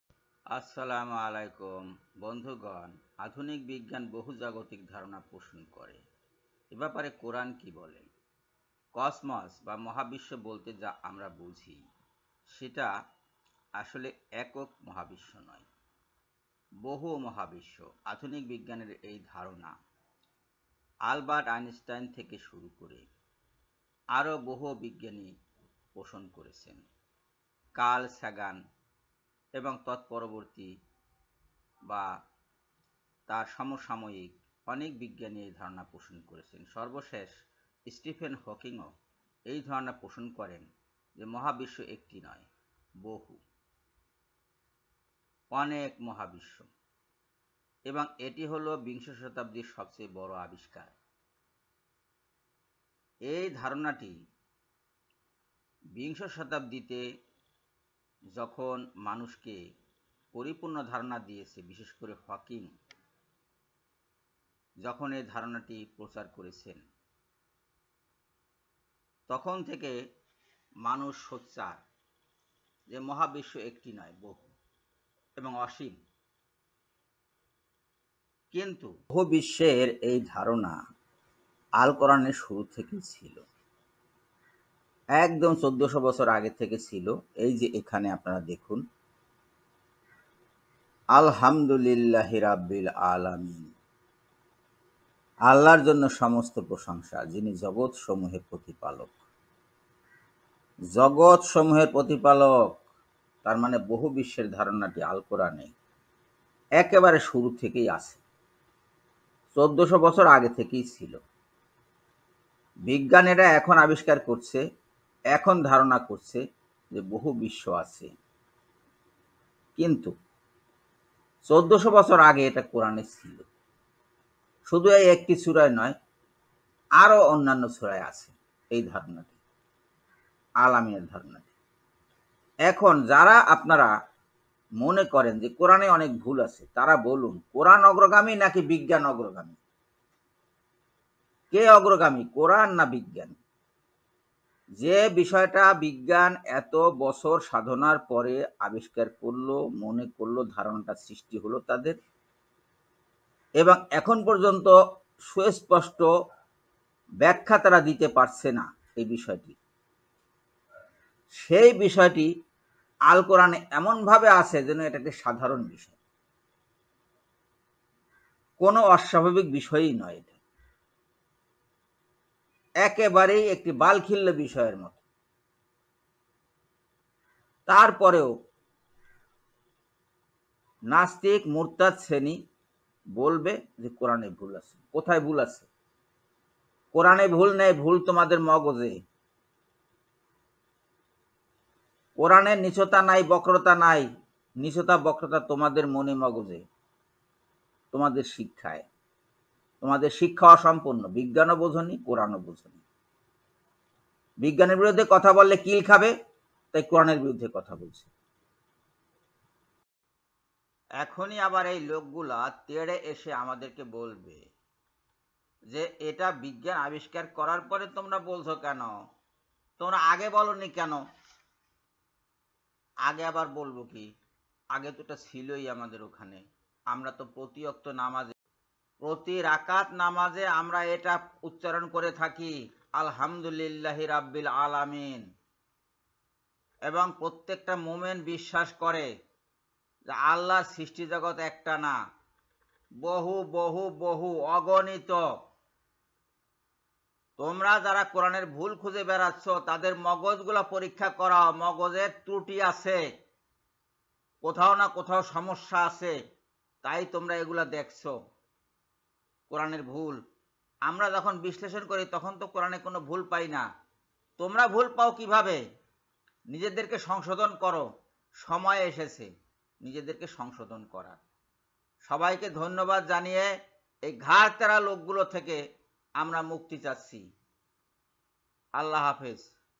বিজ্ঞানের বহু-জাগতিক ধারণা কোরআনের অনুগামী
@Affable Guide
বাংলা ভিডিও। Bangla video on scientific truth in the Quran.

ভিডিওটি দেখুন আর বন্ধুদের সাথে শেয়ার করুন।
Please, watch and share the video with your friends.